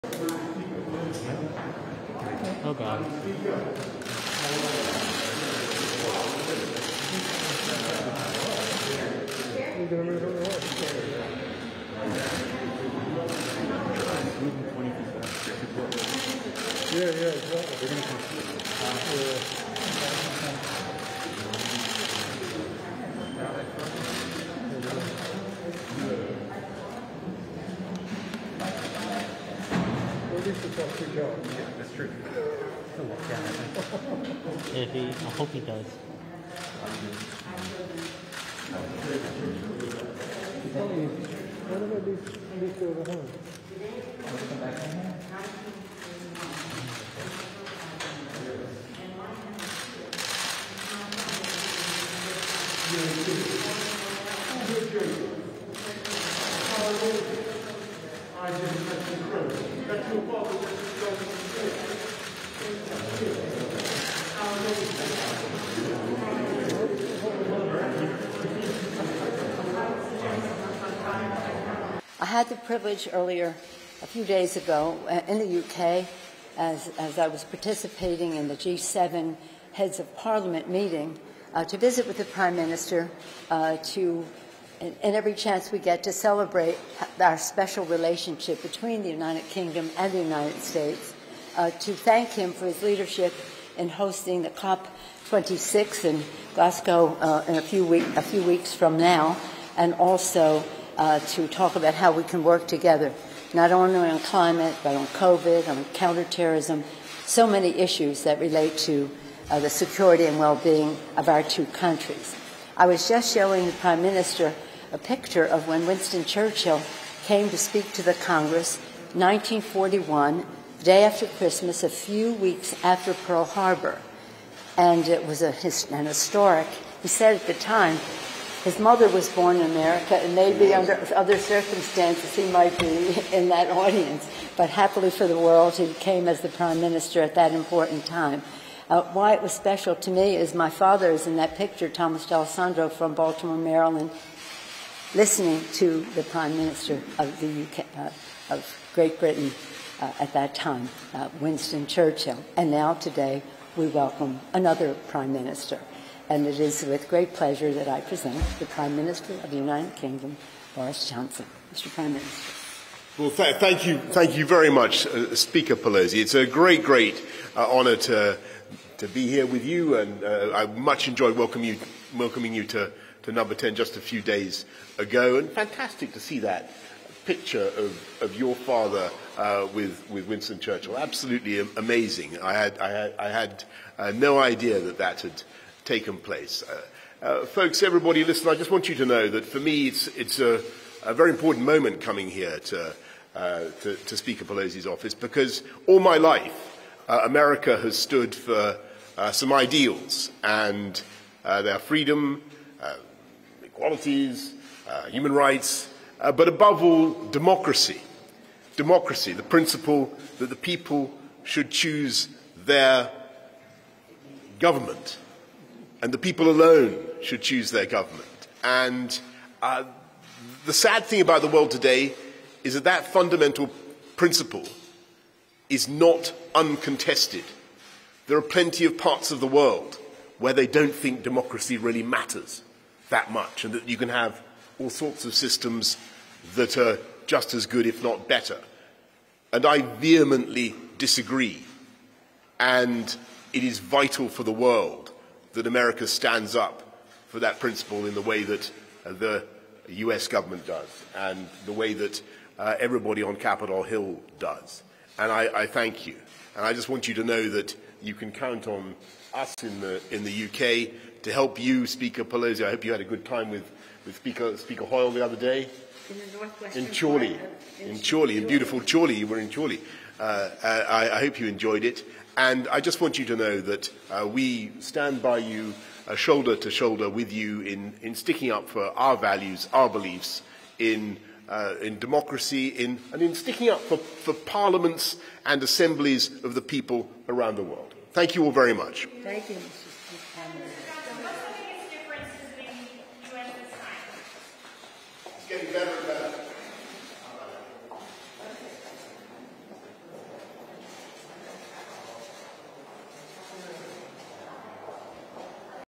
Johnson meets Pelosi, shakes hands for a minute
(22 Sep 2021) The US House of Representatives Speaker, Nancy Pelosi, met with British Prime Minister Boris Johnson on Wednesday during his visit to Washington DC.
Pelosi showed a picture of her father listening to British Prime Minister Winston Churchill at Congress in 1941.
The presentation during the photo opportunity included an unusually long handshake that endured one minute.
The Speaker thanked the prime minister for hosting the upcoming COP 26 in Glasgow.
Johnson said visiting Pelosi in her office was a "very important moment" for him as it represents common ideals and values between the two nations.